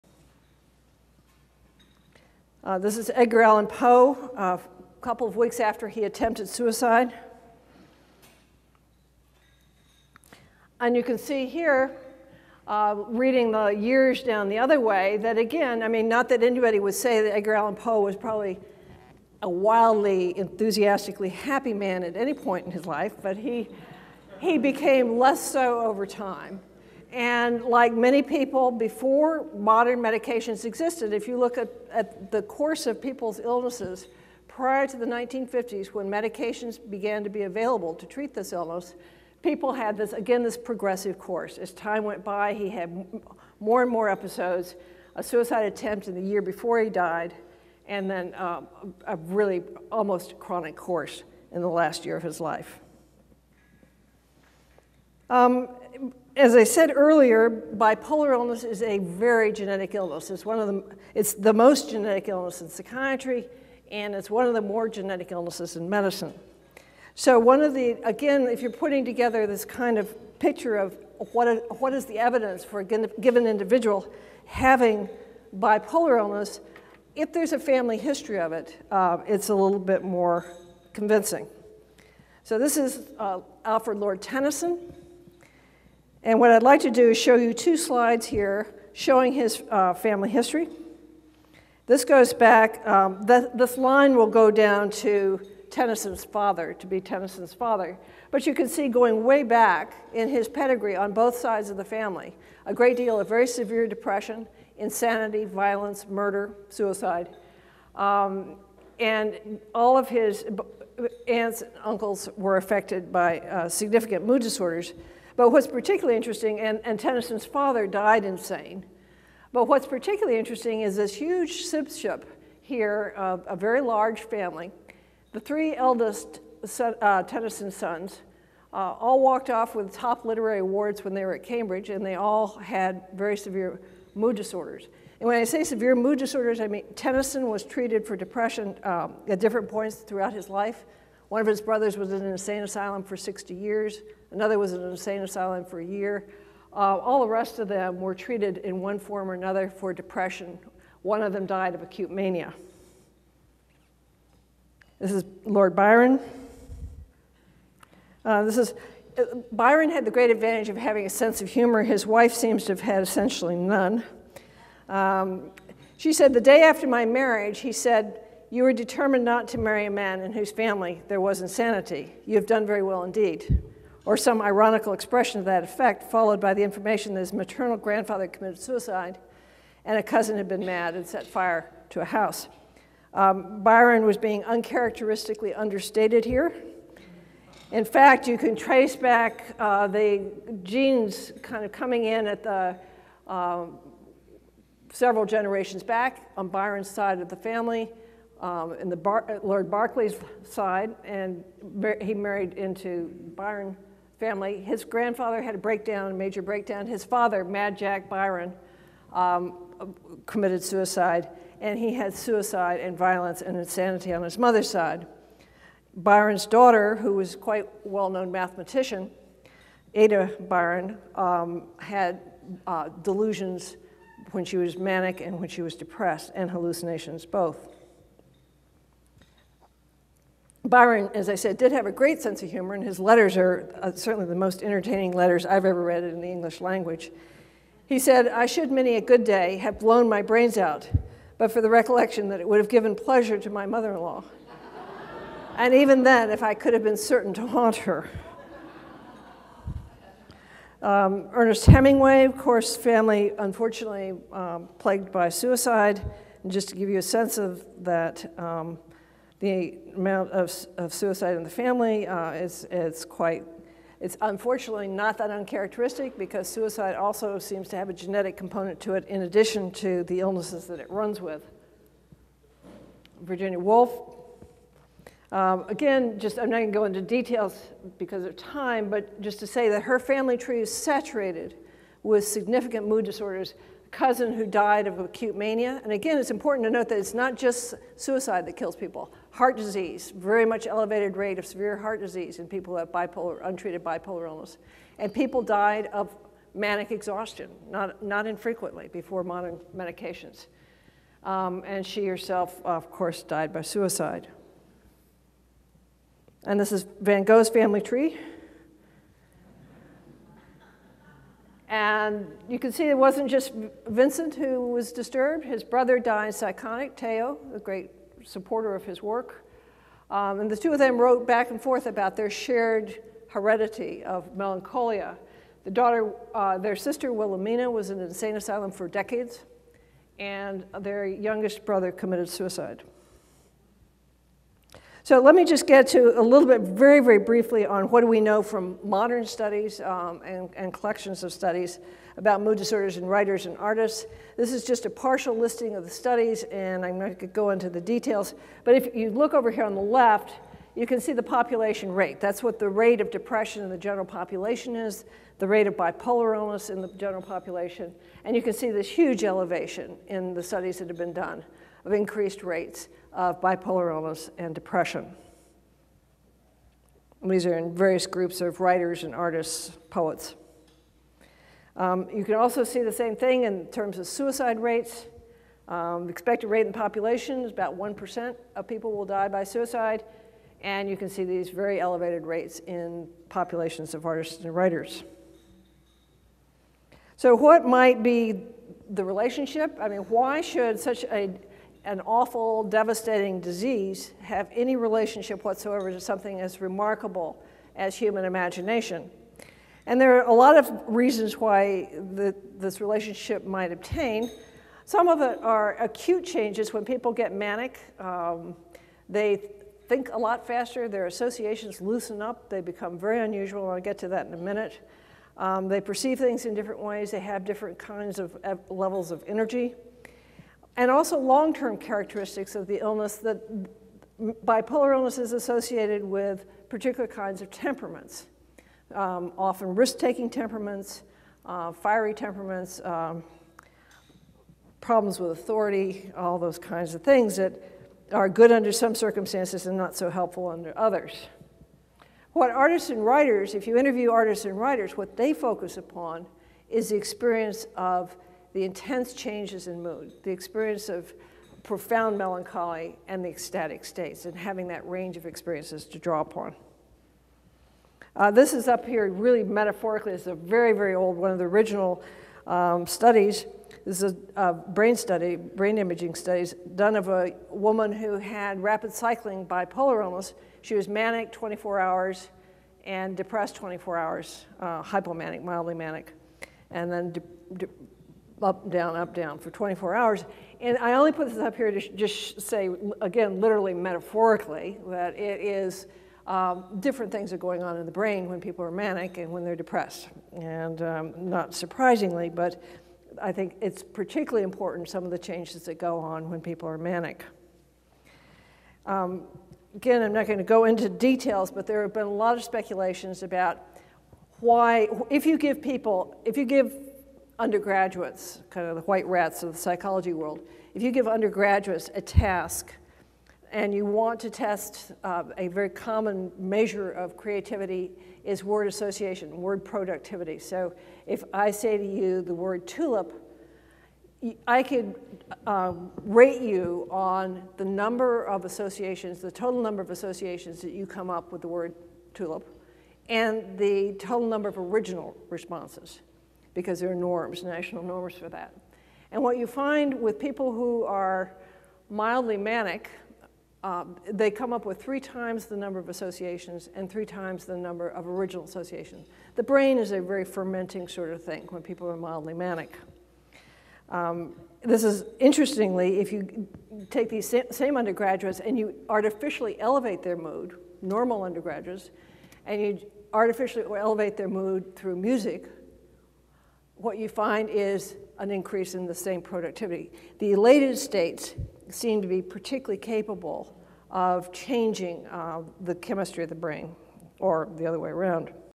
Dr. Kay Jamison Speech Part 2
Kay Redfield Jamison, Ph.D.
Professor of Psychiatry
Co-Director, Johns Hopkins, Mood Disorders Center
John Hopkins School of Medicine

"Mood Disorders and Artistic Creativity" - Part 2